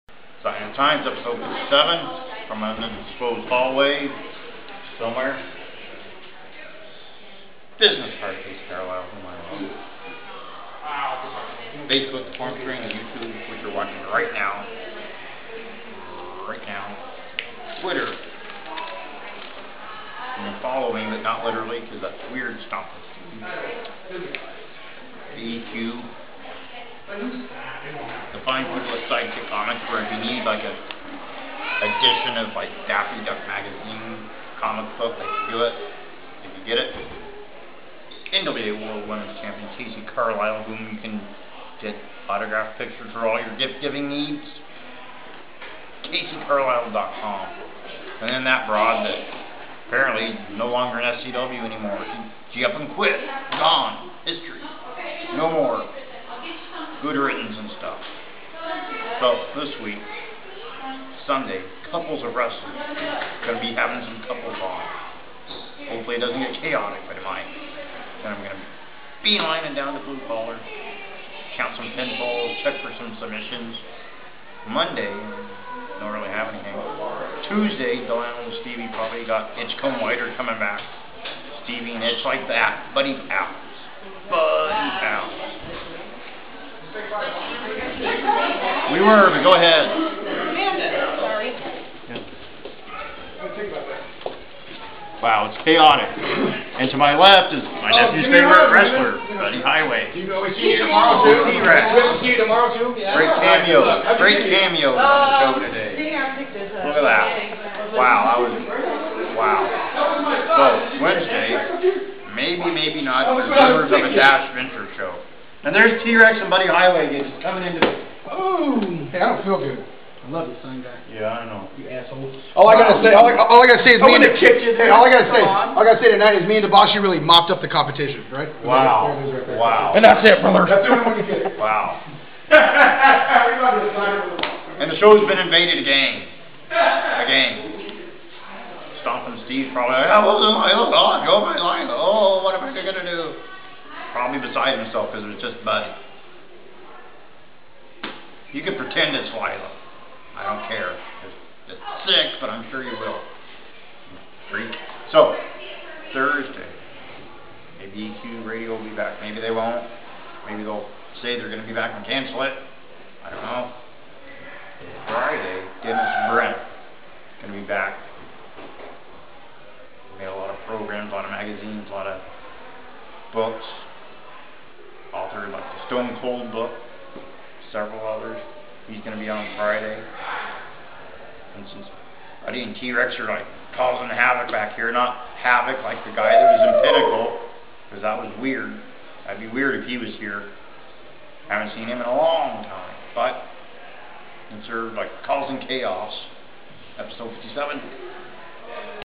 The Sign of the Times Episode 57 (The Heinz Edition)
The Sign of the Times comes to you from an undisclosed hallway. Sign Guy gives you a run down on all that's coming up, but while doing so he's got chaos coming at him. Random people, a Gresham Savior, and a zebra all come into the episode this week.